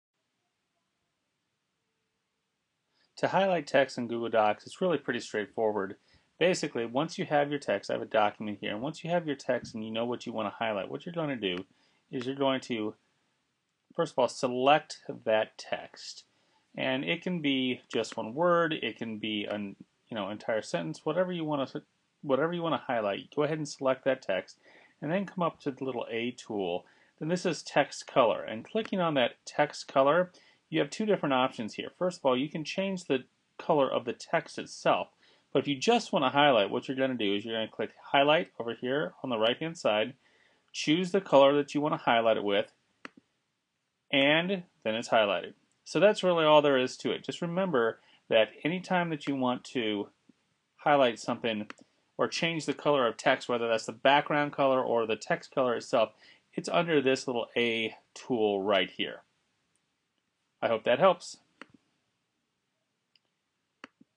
Highlighting text in Google Docs
This short video goes through the steps to highlight text in a Google Drive document.